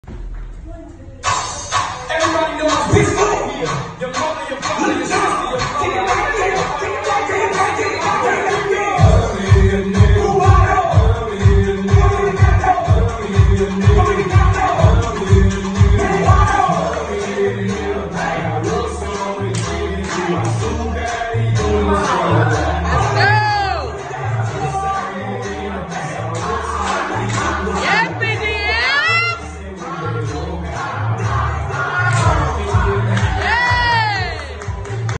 Pretty Girl Mafia Senior Squad and Makaya from DD4L
Makaya made a special trip to visit the girls and taught them this choreo. She was extremely sweet and we appreciate and love her!! Thank you Makaya and good luck at Alcoran State University!!